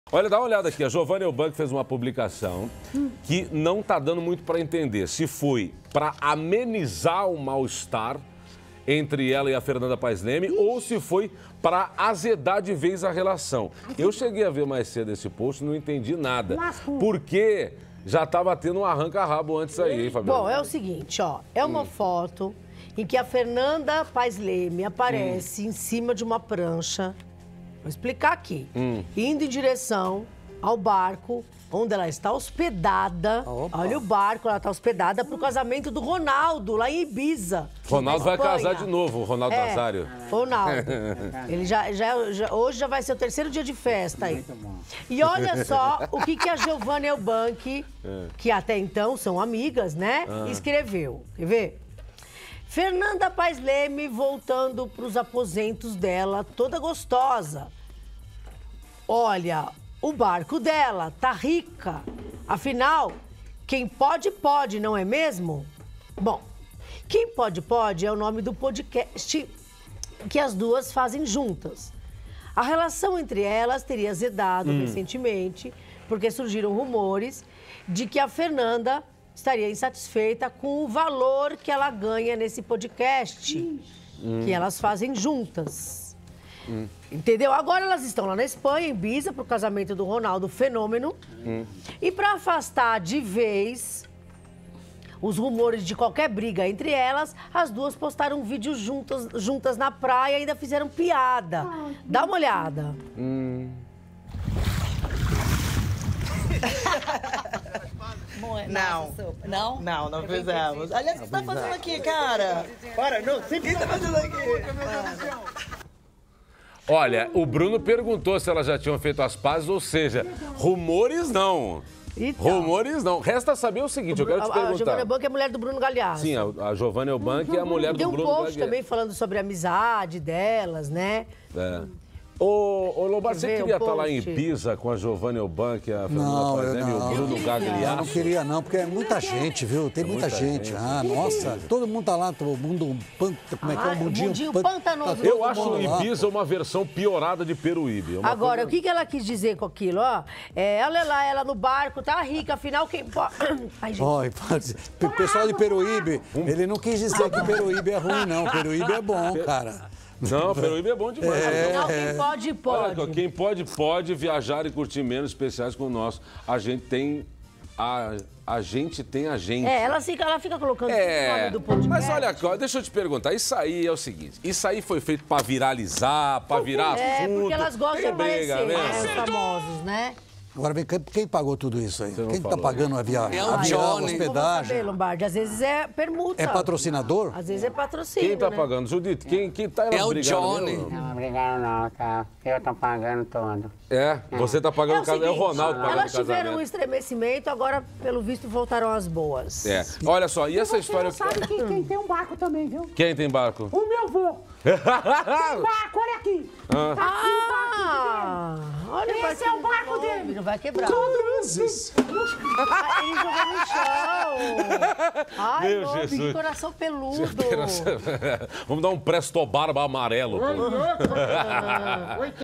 Giovanna Ewbank confunde internautas com comentário em publicação de Fernanda Paes Leme
Giovanna Ewbank fez um comentário em uma postagem de Fernanda Paes Leme que confundiu os internautas. “Voltando para os aposentos dela toda gostosa. Olha o barco dela. Tá rica. Afinal, quem pode, pode, não é mesmo?” escreveu Giovanna, em uma foto em que Fernanda aparece em uma prancha indo ao barco onde está hospedada para o casamento de Ronaldo. “Quem pode, pod” é o nome do podcast delas. A relação delas teria azedado recentemente, depois que surgiram rumores de que Fernanda estaria insatisfeita com o que ela ganha no podcast.